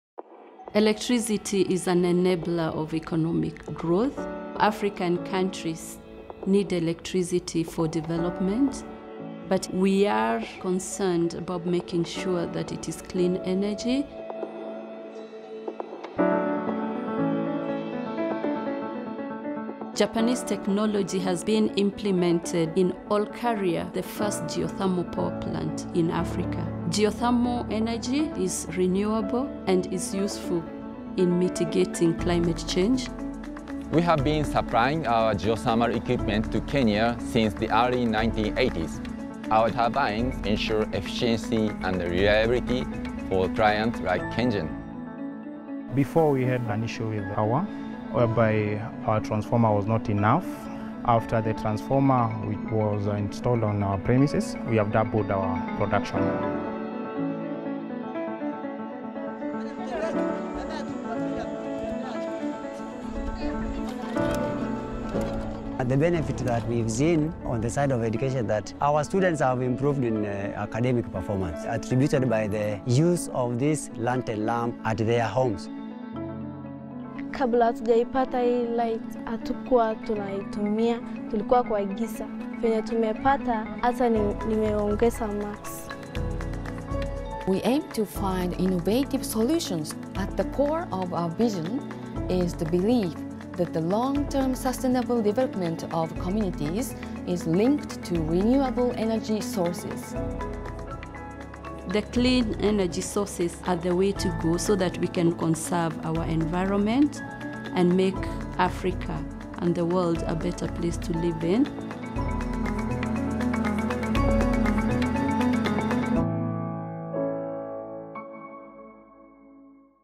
Sharing Solutions: Mitigating Climate Change
Japanese technologies provide innovative solutions to mitigate climate change, working together with people and businesses in other countries. Taking up a geothermal power generation system and solar lanterns in Kenya, we introduce Japan's innovative efforts toward clean and reliable energy through international cooperation, which leads to sustainable development of communities.